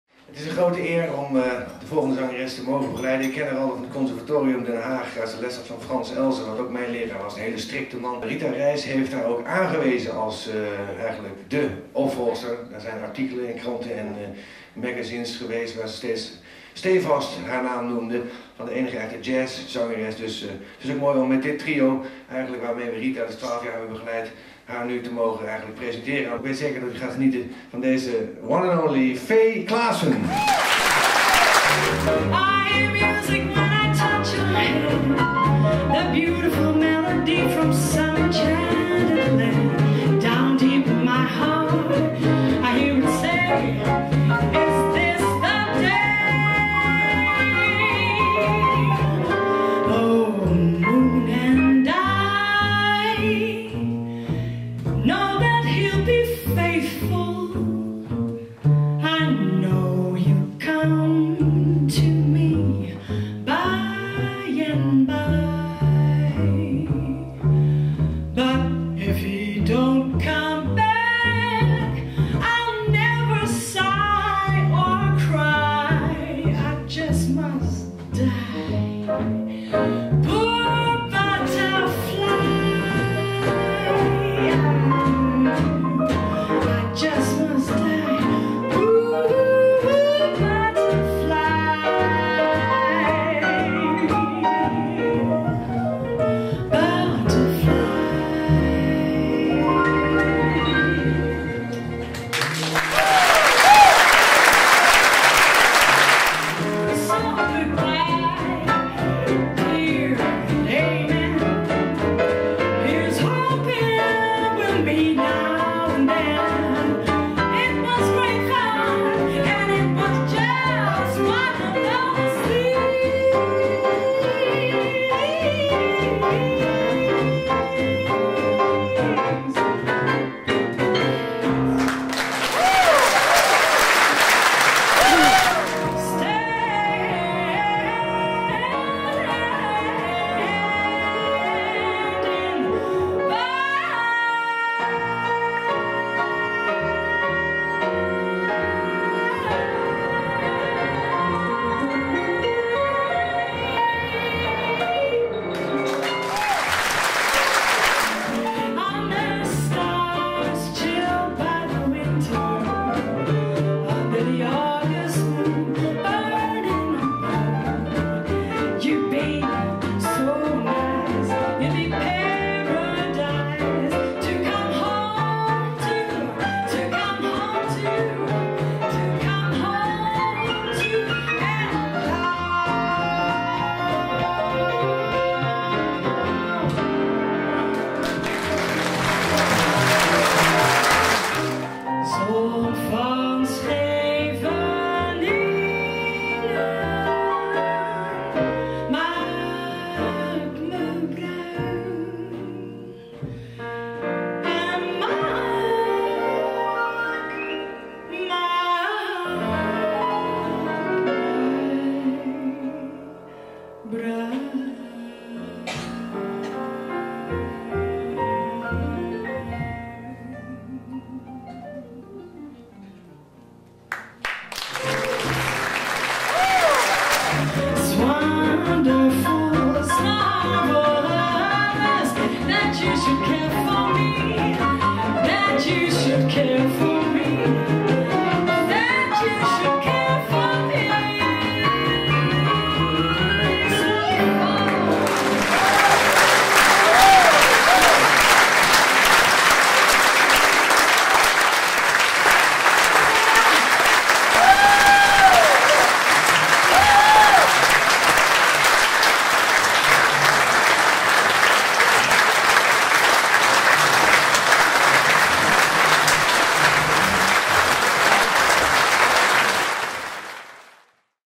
Fay Claassen invited by Trio Peter Beets - short compilation
Concertgebouw Amsterdam
08 juli 2014